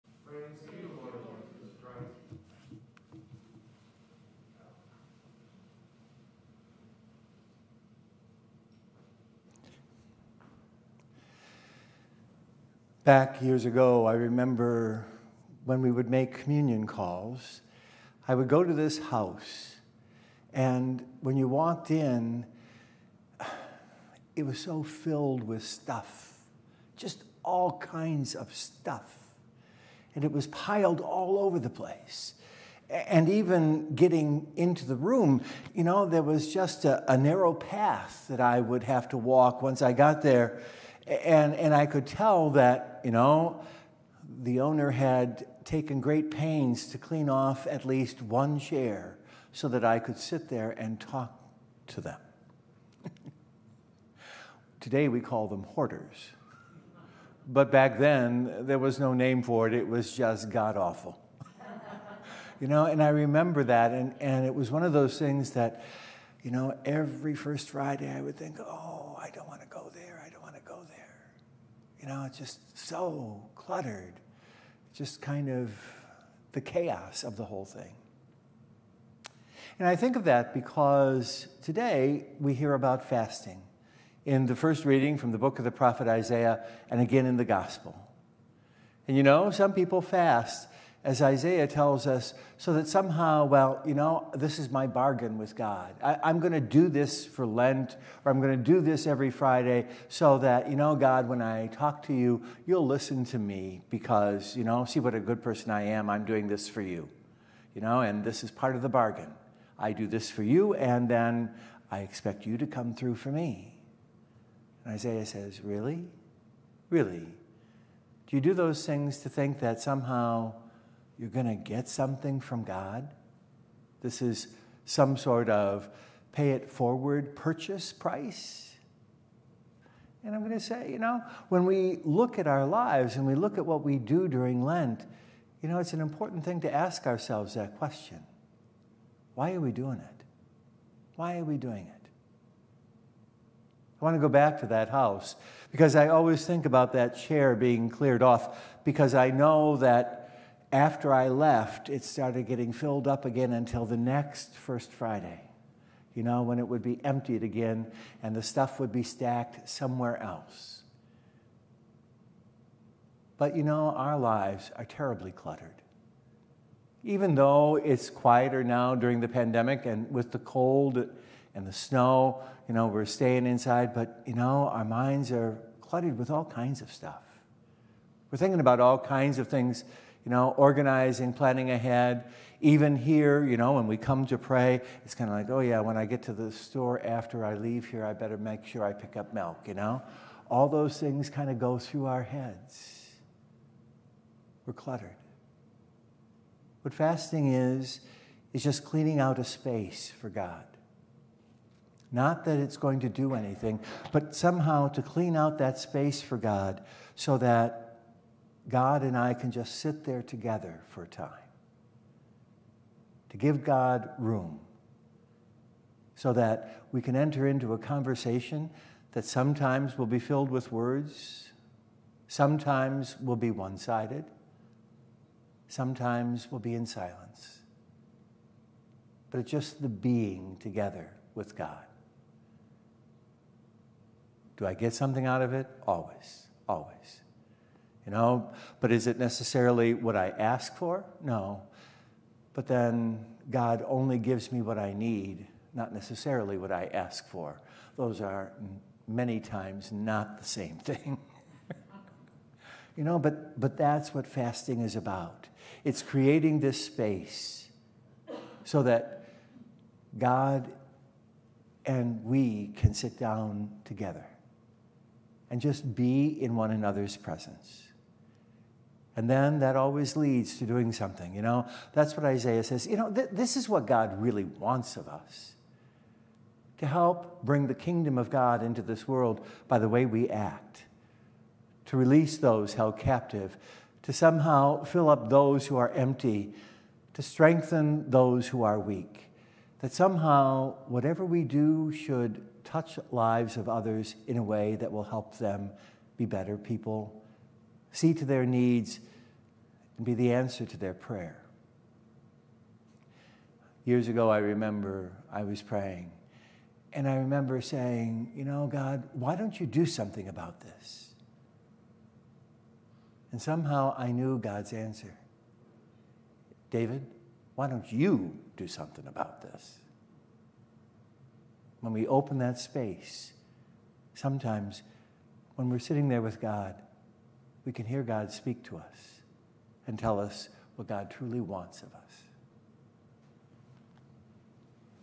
Homily 2-19
Friday Mass, live broadcast from St Mark the Evangelist Catholic Church